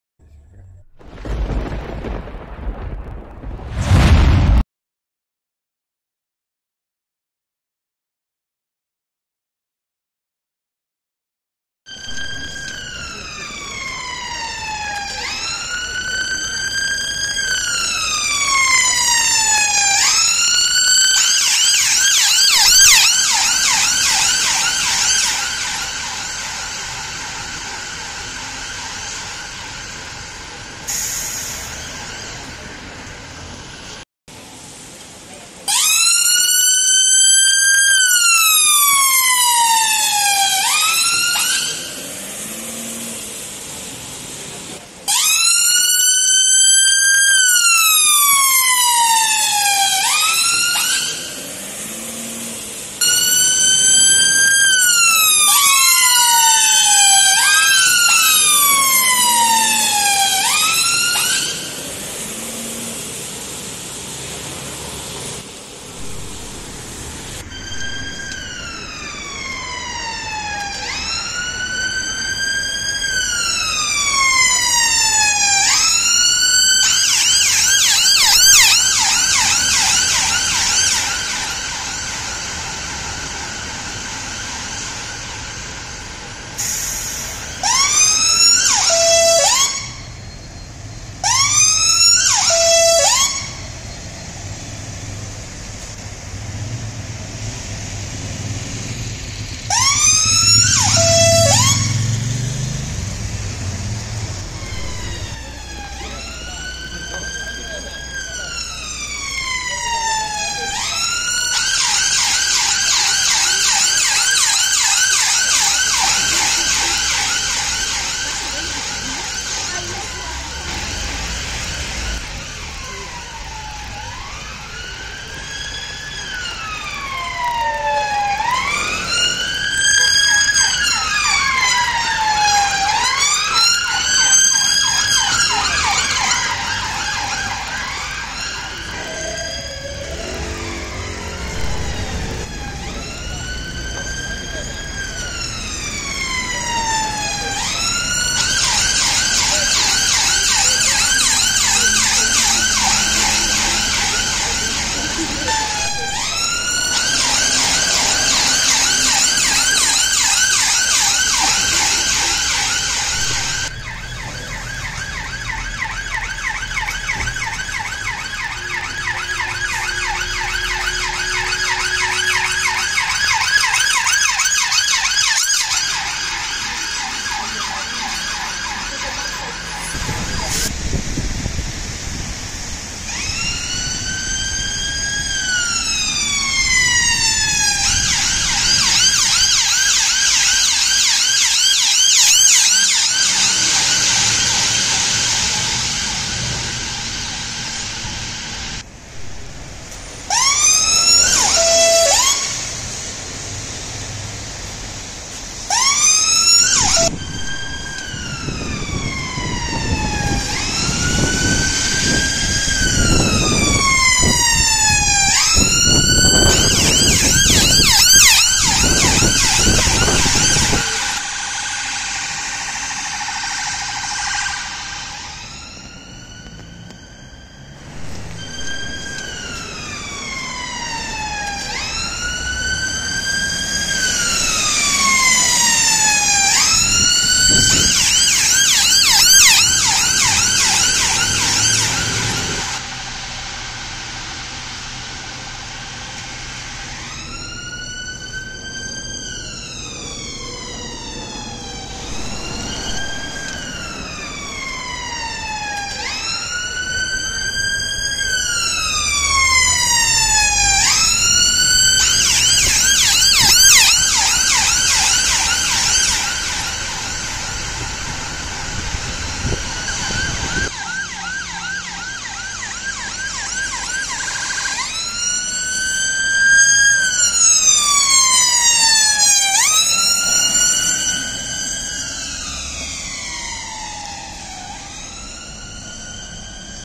London Ambulance Car Sound Effect to make your dogs barking.
London Ambulance Car Sound Effect Compilation to make your dogs barking.